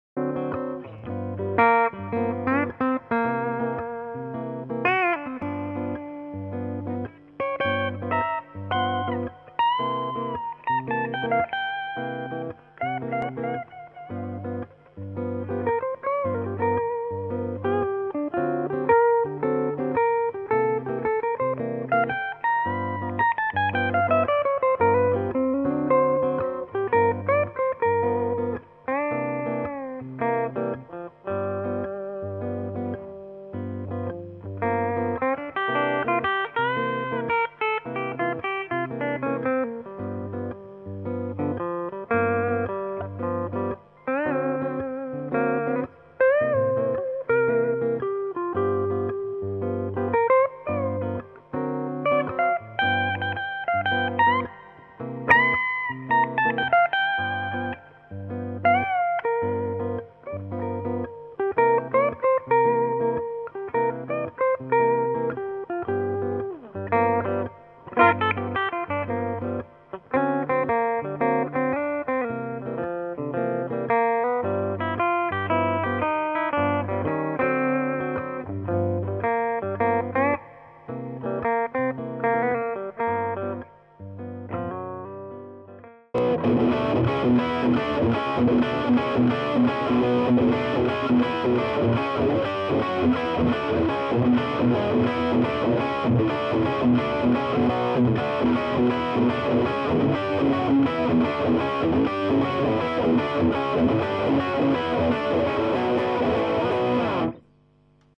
Inspector Russian Guitars (Inspector TT-3)
Гитара российского производства Inspector TT-3. Корпус сделан из 3 кусков ясеня, покрыт маслом и воском. Способ обработки дерева: браширование. Гриф: клен, накладка: клен, матовый полиуретановый лак, профиль С, 22 лада. Звукосниматель и фурнитура: КНР.